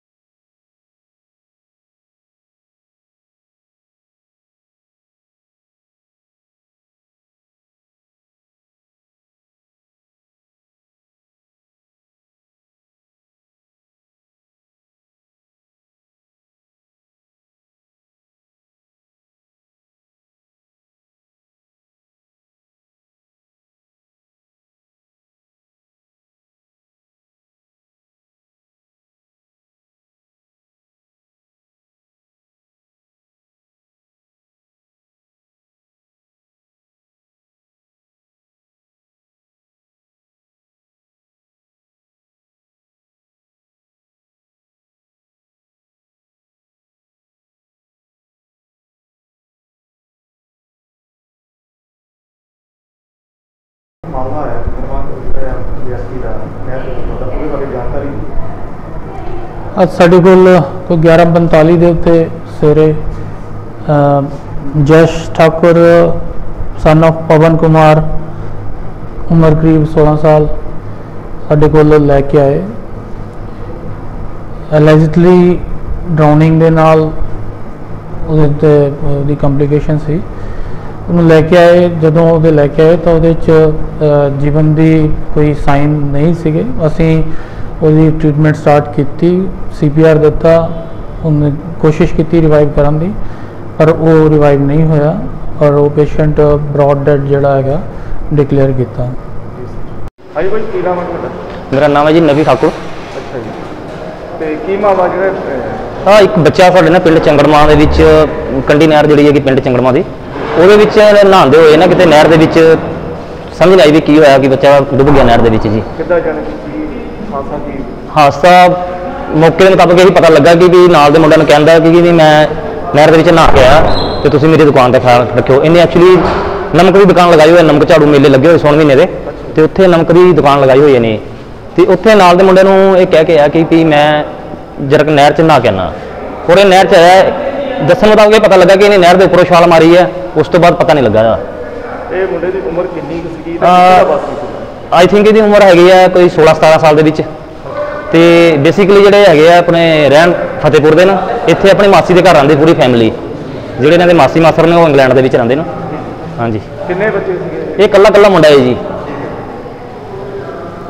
ਤਲਵਾੜਾ ਦੇ ਪਿੰਡ ਚੰਗੜਵਾਂ ਵਿਖੇ ਨਹਿਰ ਵਿੱਚ ਡੁੱਬਣ ਨਾਲ ਹੋਈ 16 ਸਾਲ ਦੇ ਨੌਜਵਾਨ ਦੀ ਮੌਤ ਮੇਲੇ ਵਿੱਚ ਲਾਈ ਸੀ ਦੁਕਾਨ।
Talwara Talwara news#Mukerian news#Hoshiatpur#Punjab news#